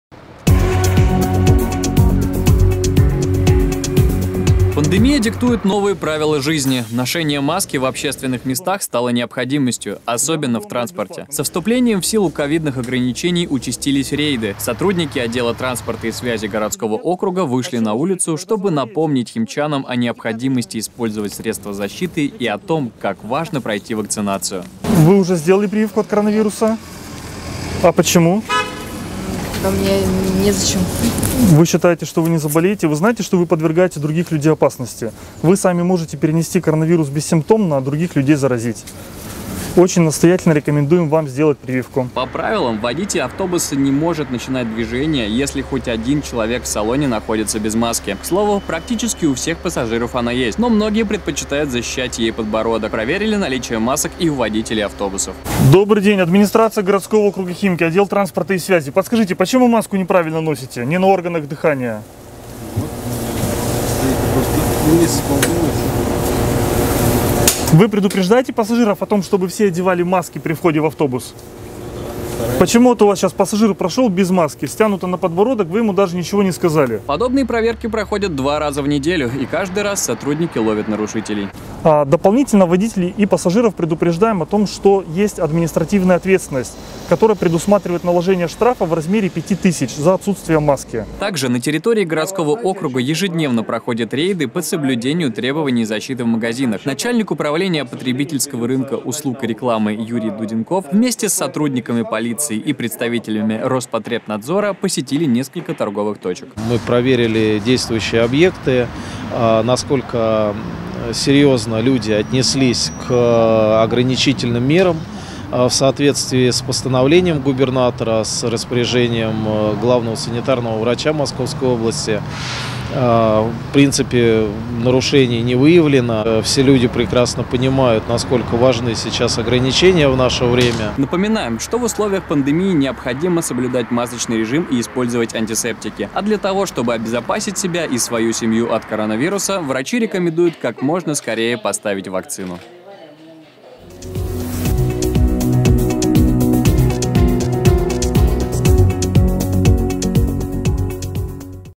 В Химках провели масочный рейд в общественном транспорте и в торговых центрах. 3.11.2021
Пандемия диктует новые правила жизни. Ношение маски в общественных местах стало необходимостью. Особенно в транспорте. Со вступлением в силу ковидных ограничений участились рейды. Сотрудники отдела транспорта и связи городского округа вышли на улицы, чтобы напомнить химчанам о необходимости использовать средства защиты и о том, как важно пройти вакцинацию.